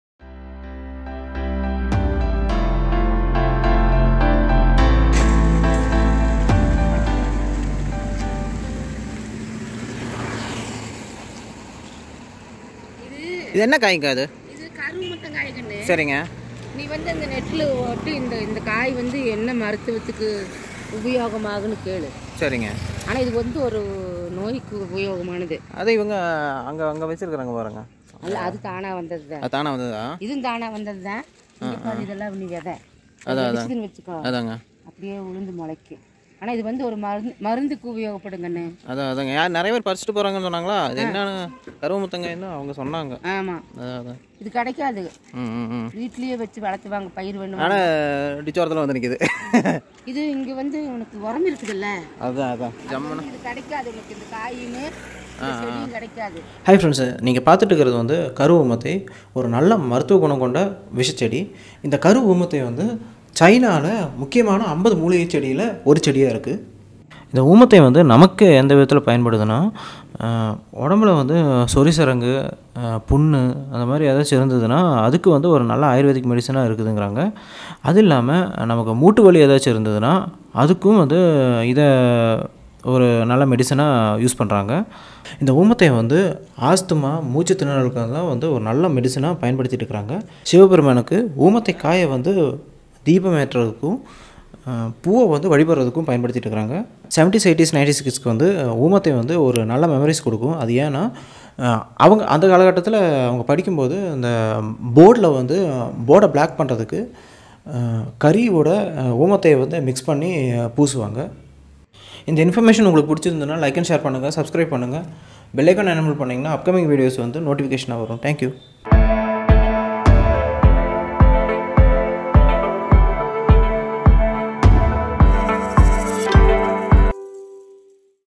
கரு ஊமத்தை as Datura Metel | Few Info
கரு ஊமத்தை as Datura Metel which is poisonous plant but which is used as Ayurvedic medicine for some asthma and breathing problem. கரு ஊமத்தை which is used as remedy for knee pain and also கரு ஊமத்தை used for deepam to the lord Shiva and flower used for Pooja to lord shiva.